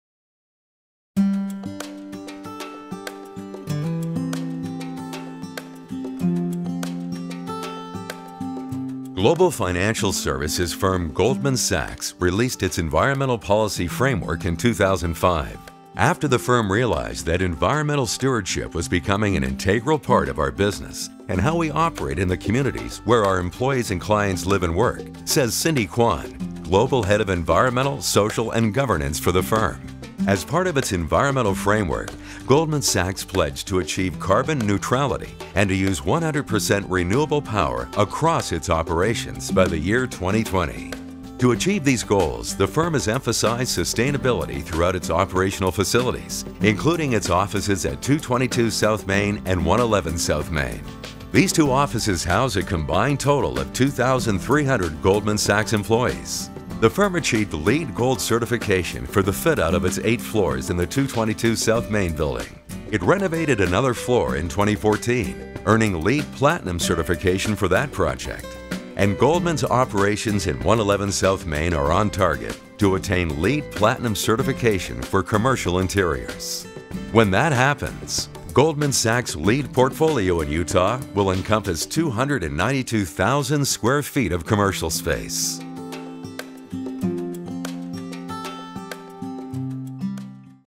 The Goldman Sachs Group: Green Business Awards
Congratulations to The Goldman Sachs Group, honored for "Corporate Excellence" at Utah Business' 2017 Green Business Awards.